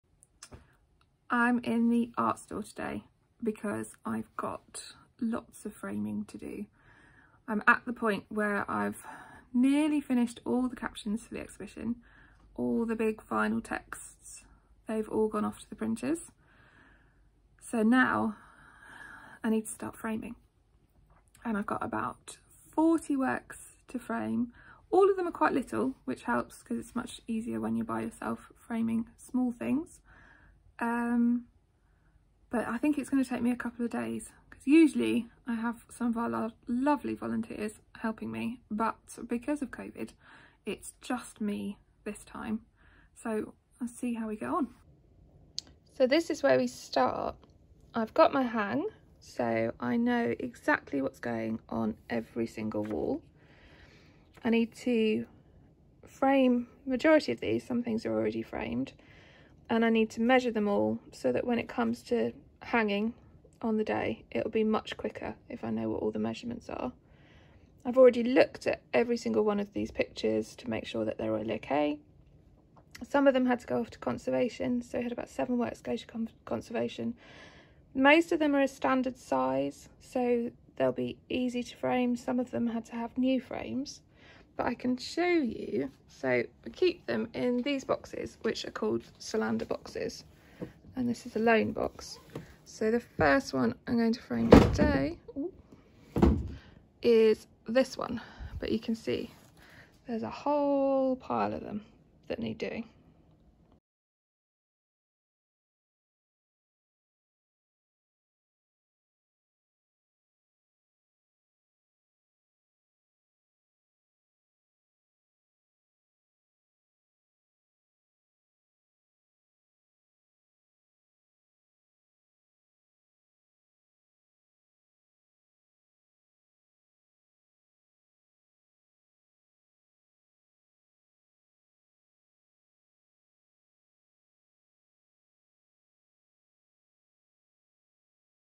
Framing in the Art Store
Victoria Partridge, Keeper of Fine and Decorative Arts at The Higgins Bedford, takes us into the art store and shows us her work on framing the paintings and prints for the 'Going to Town - 200 Years of Bedford's Art' exhibition, opening 10 July.